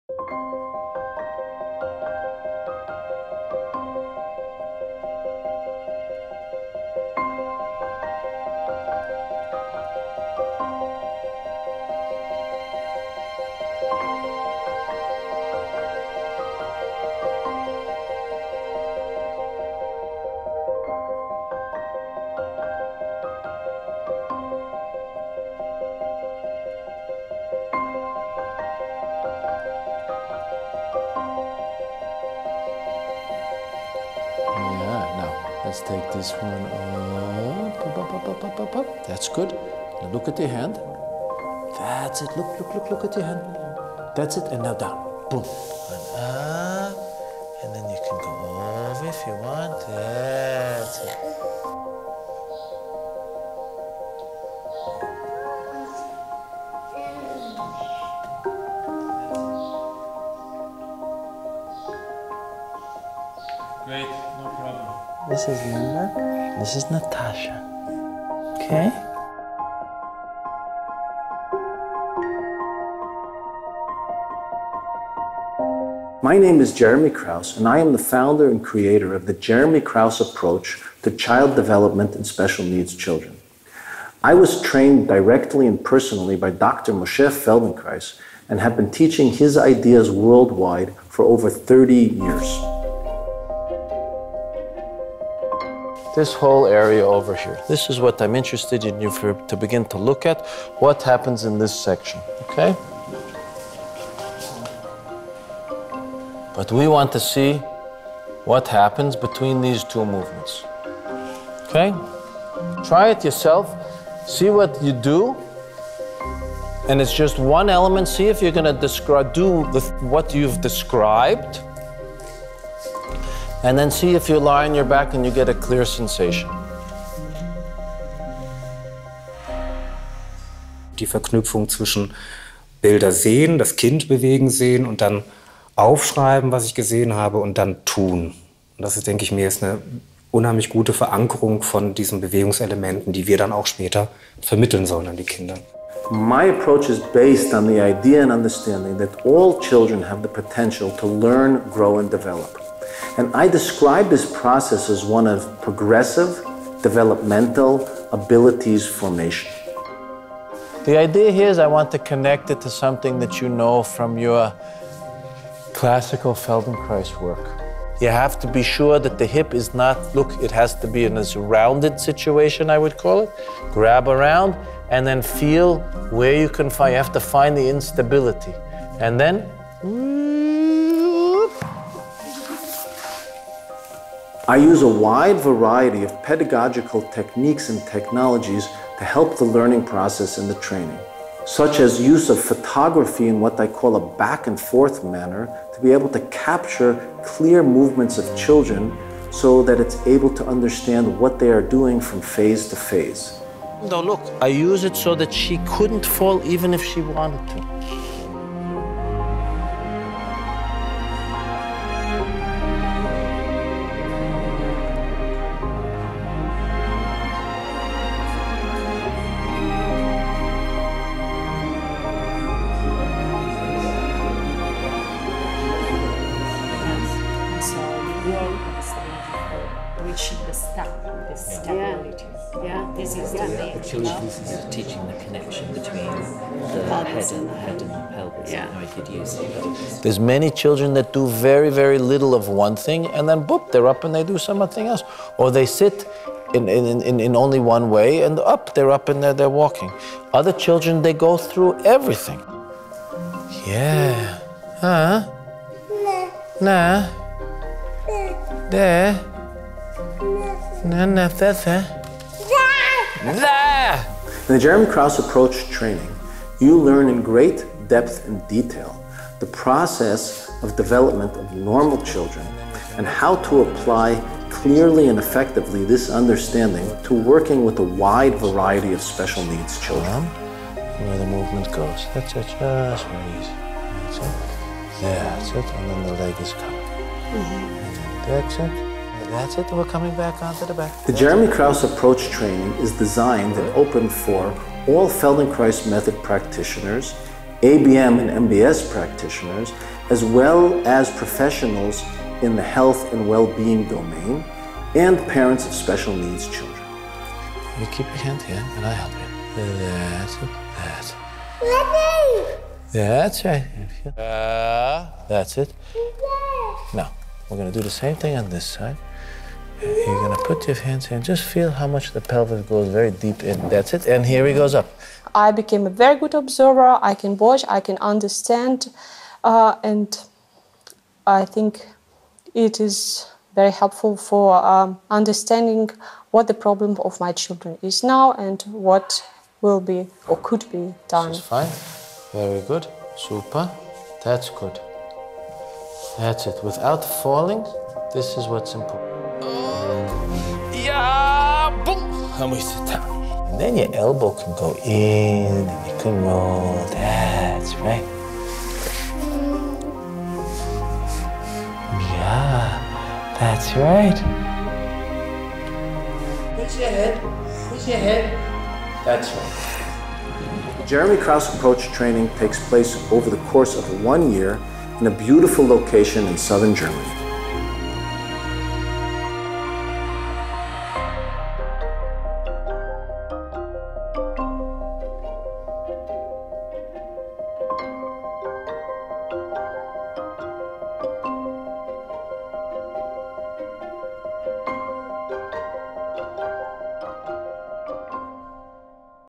Jeremy Krauss Approach to child development and Special Needs Children
A short film about the JKA training. The Jeremy Krauss Approach to child development and Special Needs Children is a unique way of helping Special Needs Children learn, grow and develop based on the work of Dr. Feldenkrais